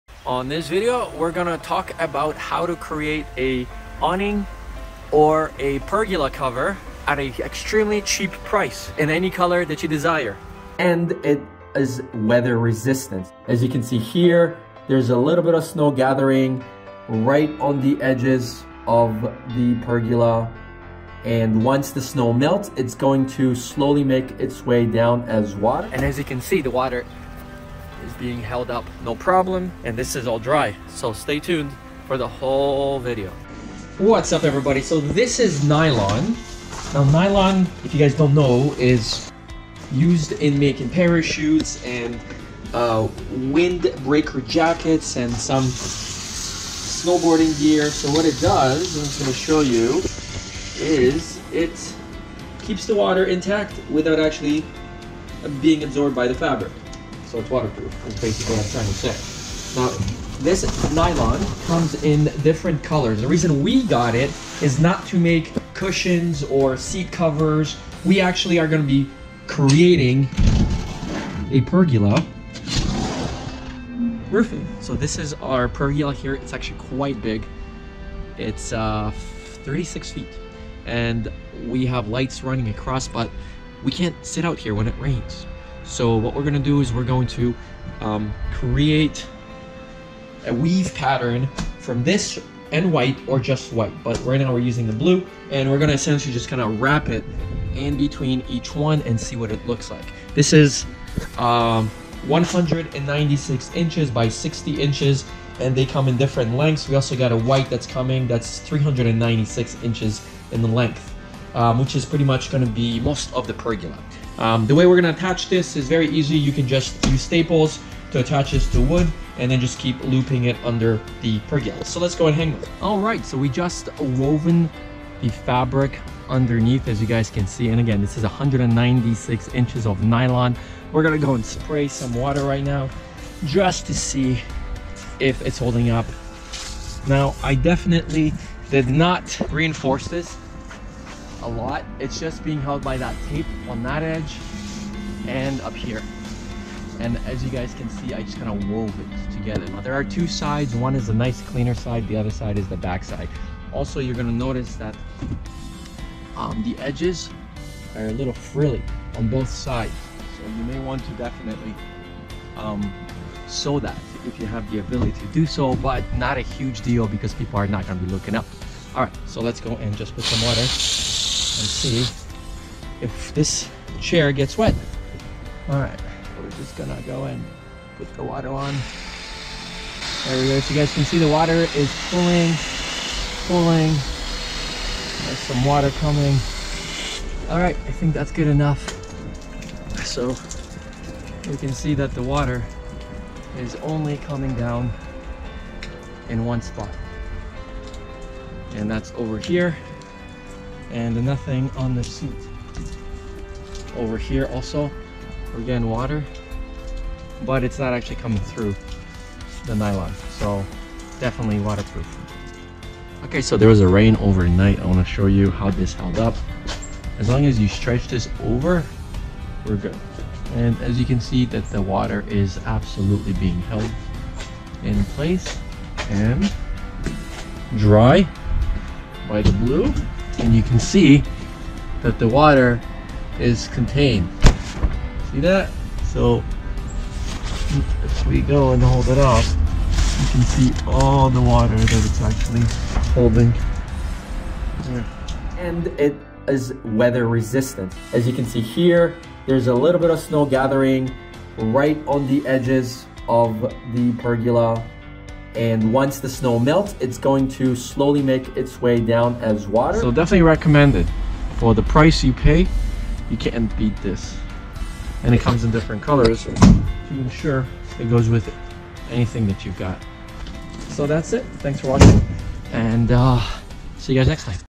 Waterproof DIY Patio Cover Roof / Pergola Cover / Canopy Cover - 9KM DWLIFE 40D Nylon
- This is a snow proof and waterproof DIY Nylon that we used to create a 40 foot Pergola Covering, patio cover roof in our outdoor space. It will not tear, its airtight and you can use it for tents, tarps, flags, but it will fade slightly in the sun over time. It can be used to Patio Covers, awnings, chair covers, anything else you can sew. Diy Tarp Canopy.  9KM DWLIFE Ripstop Nylon White Fabric 60"x196"(WxL) 40D Waterproof Durable Lightweight Airtight Fabric for Kite Inflatable Flag Tarp Cover Tent DIY Projects, comes as 10 or 5 Meter lenths at 60" wide and comes in many colors.PU Coating gives fabric excellent water repellent, UV resistance and a protective layer to maintain the bright colors.